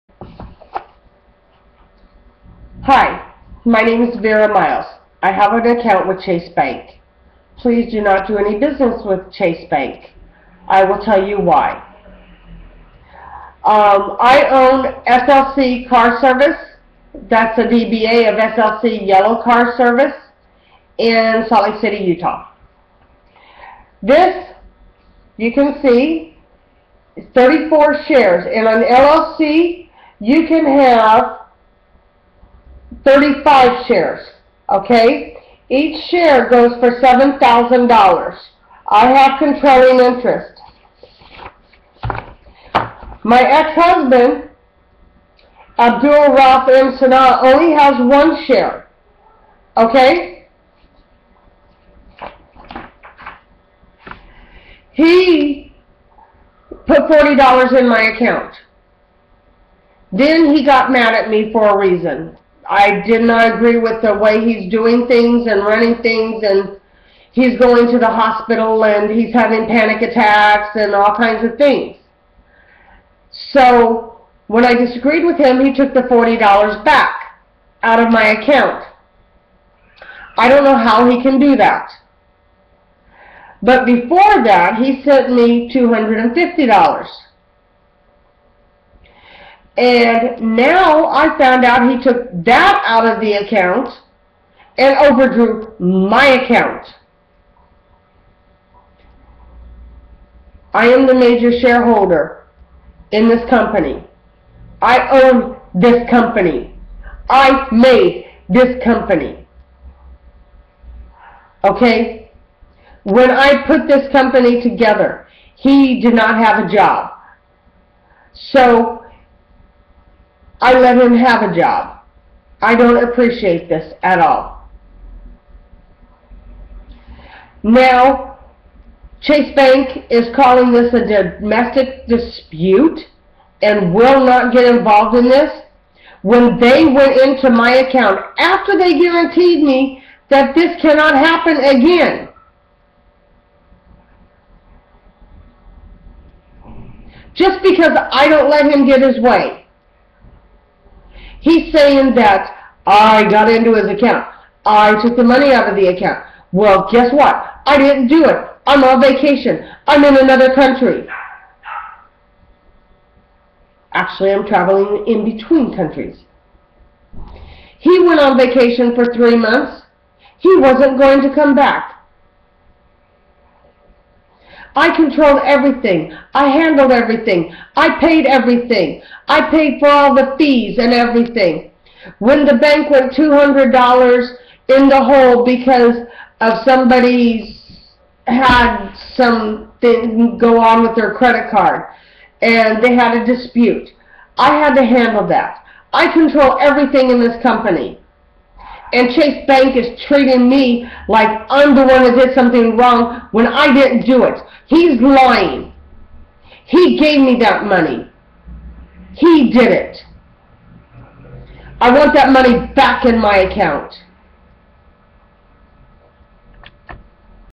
DO NOT DO BUSINESS WITH CHASE BANK
MY EX-HUSBAND AND I OWN A COMPANY HE PUT MONEY IN MY ACCOUNT AND WHEN HE GOT PISSED OF BECAUSE I DISAGREED WITH HIM HE TOOK THE MONEY BACK.